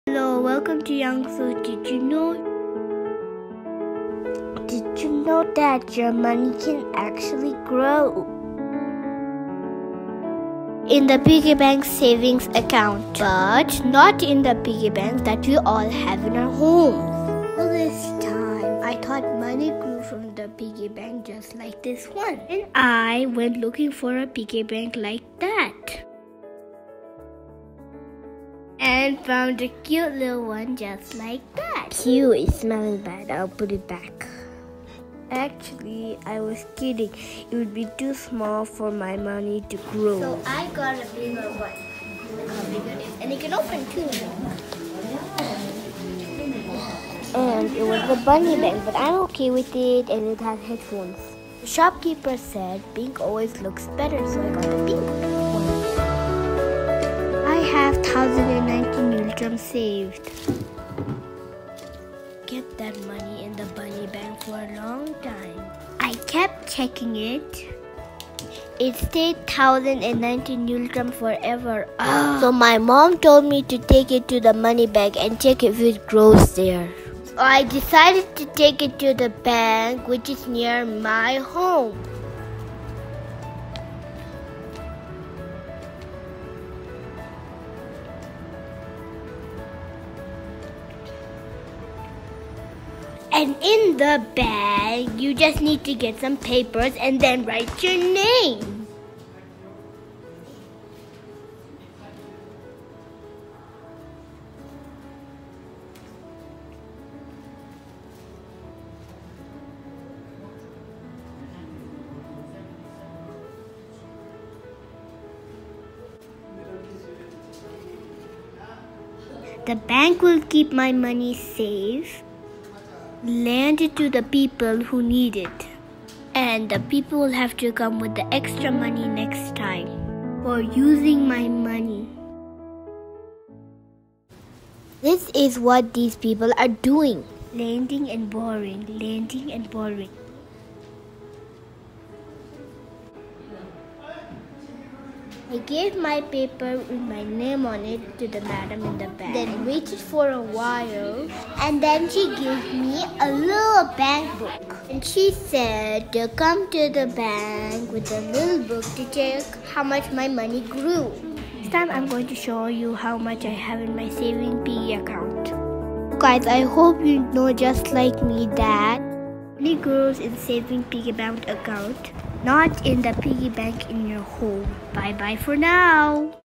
YangtshoDelma DidYouKnow Episode
DidYouKnow that money 💰 💴 💵 could actually grow 😃, but we need to keep it in the right place.
Bhutan National Bank Limited Royal Monetary Authority Awesomekids